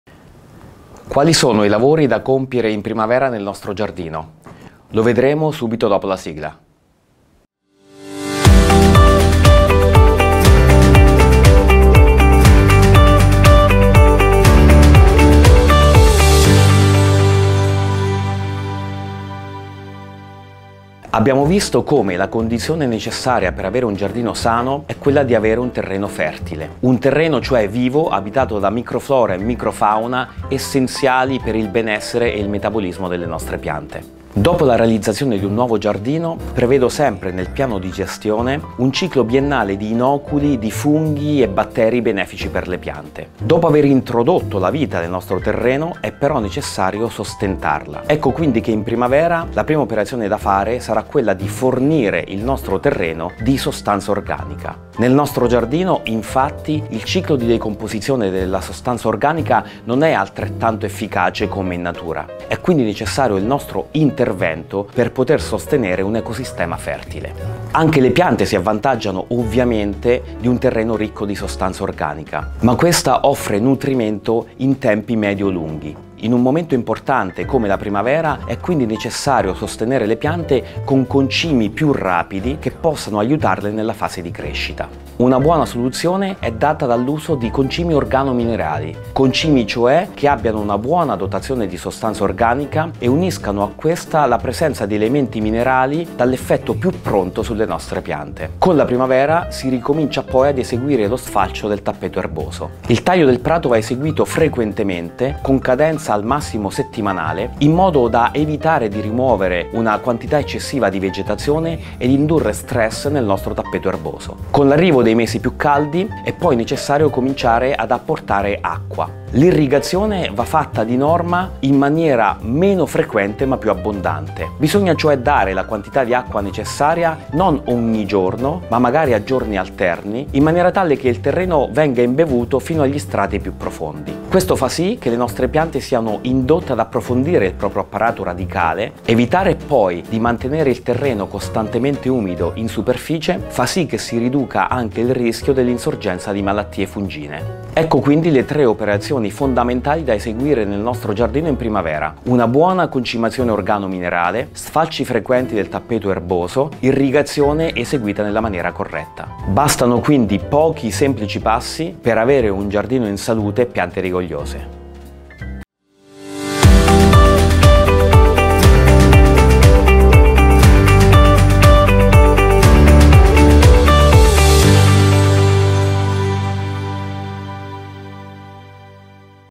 La primavera (seconda parte)
Abbiamo visto come la condizione necessaria per avere un giardino sano è quella di avere un terreno fertile, un terreno cioè vivo, abitato da microflora e microfauna essenziali per il benessere e il metabolismo delle nostre piante.

Dopo la realizzazione di un nuovo giardino prevedo sempre, nel piano di gestione, un ciclo biennale di inoculi di funghi e batteri benefici per le piante.

Dopo aver introdotto la vita nel nostro terreno è però necessario sostentarla.

Ecco quindi che in primavera la prima operazione da fare sarà quella di fornire il nostro terreno di sostanza organica.

Nel nostro giardino infatti il ciclo di decomposizione della sostanza organica non è altrettanto efficace come in natura.
E’ quindi necessario il nostro intervento per poter sostenere un ecosistema fertile.

Anche le piante si avvantaggiano ovviamente di un terreno ricco di sostanza organica, ma questa offre nutrimento in tempi medio-lunghi.

In un momento importante come la primavera, è quindi necessario sostenere le piante con concimi più rapidi che possono aiutarle nella fase di crescita.

Una buona soluzione è data dall'uso di concimi organo-minerali: concimi cioè che abbiano una buona dotazione di sostanza organica e uniscano a questa la presenza di elementi minerali dall'effetto più pronto sulle nostre piante.

Con la primavera si ricomincia poi ad eseguire lo sfalcio del tappeto erboso.

Il taglio del prato va eseguito frequentemente, con cadenza al massimo settimanale, in modo da evitare di rimuovere una quantità eccessiva di vegetazione e indurre stress nel nostro tappeto erboso.

Con l'arrivo dei mesi più caldi è poi necessario cominciare ad apportare acqua.

L’irrigazione va fatta di norma in maniera meno frequente ma più abbondante: bisogna cioè dare la quantità di acqua necessaria non ogni giorno, ma magari a giorni alterni in maniera tale che il terreno venga imbevuto fino agli strati più profondi.

Questo fa sì che le nostre piante siano indotte ad approfondire il proprio apparato radicale.

Evitare poi di mantenere il terreno costantemente umido in superficie fa sì che si riduca anche il rischio dell'insorgenza di malattie fungine.

Ecco quindi le tre operazioni fondamentali da eseguire nel nostro giardino in primavera:
una buona concimazione organo-minerale;
sfalci frequenti del tappeto erboso;
irrigazione eseguita nella maniera corretta.

Bastano quindi pochi semplici passi, per avere un giardino in salute e piante rigogliose.